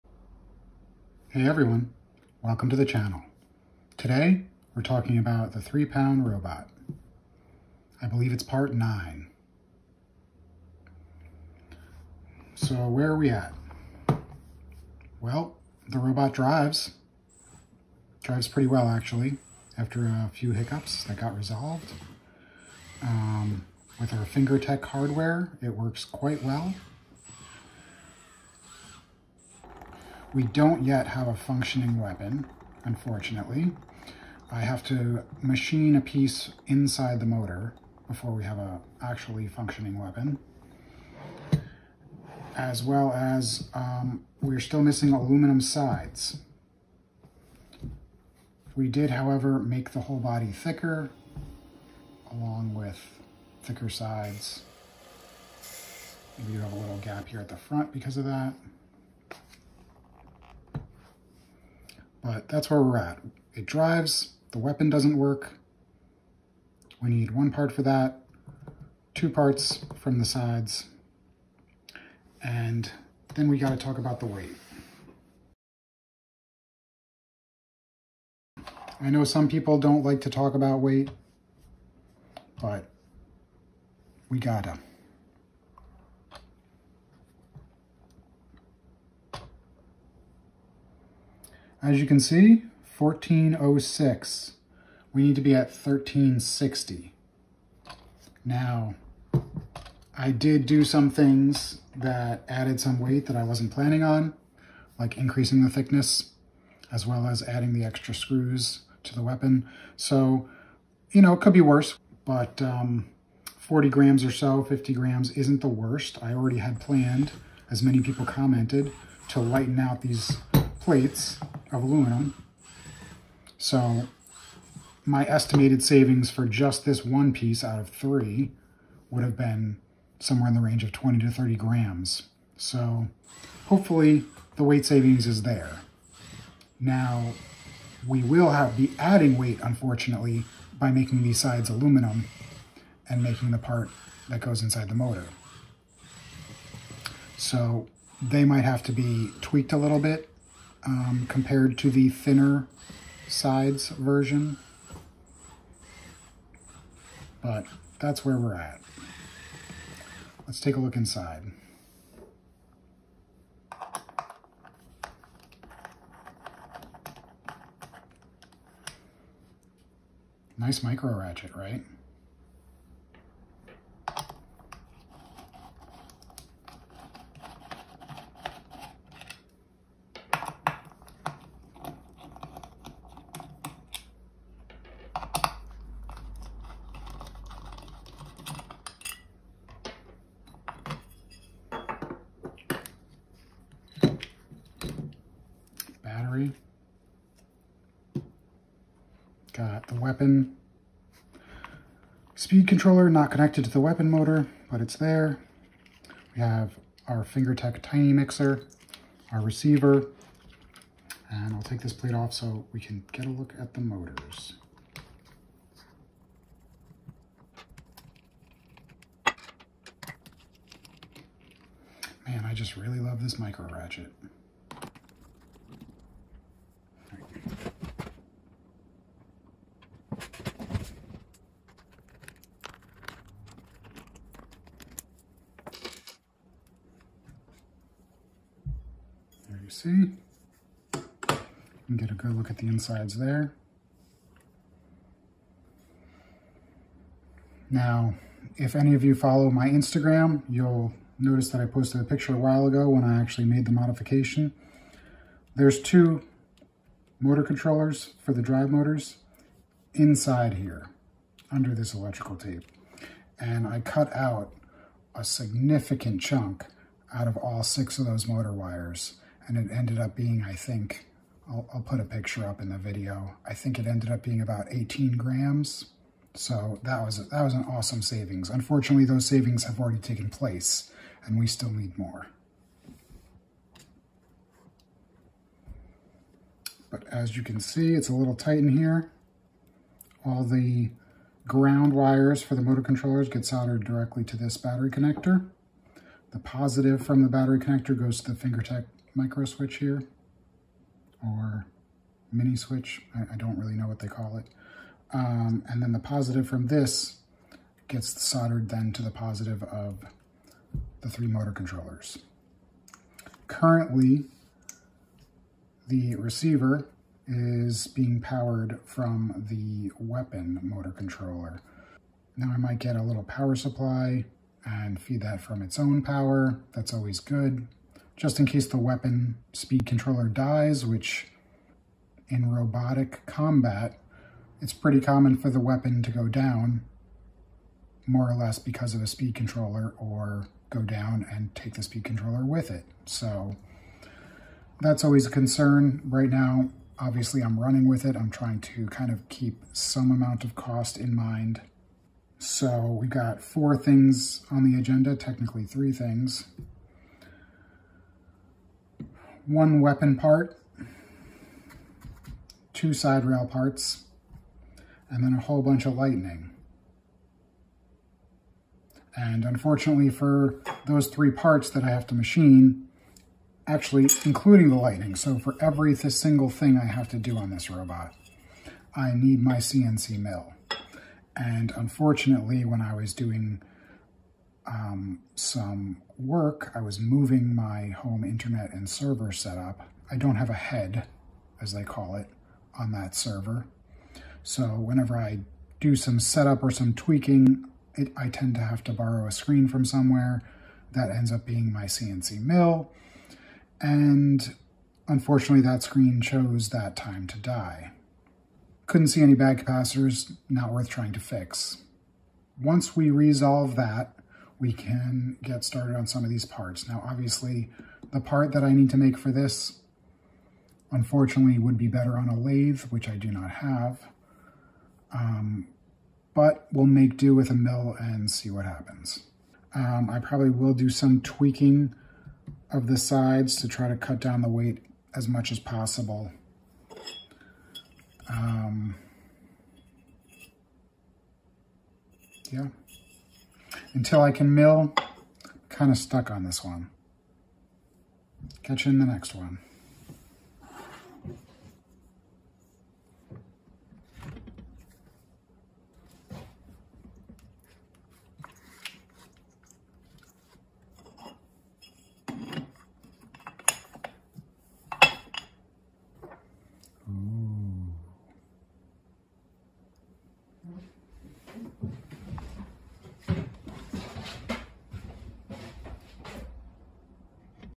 Status Update (3lb Robot - Part 9)
Hey everyone, today we are going over the current status of the 3lb Robot build.

This is part of a series starting from a concept, then building a 3lb combat robot, sometimes called beetle-weight, and taking it to compete at NHRL.

Fiberglass FR4 2mm
Fiberglass FR4 3mm
Overture High Speed TPU Filament 1.75mm:
Brushless Outrunners 3530:
Weap ESC
Flysky 3ch Receiver
Hardware:
Stainless M6x12mm Flat
Stainless M6x12mm Button
Stainless M6 Nuts
Stainless M4x12mm Button
Stainless M4x10mm Flat
Stainless M4 Nuts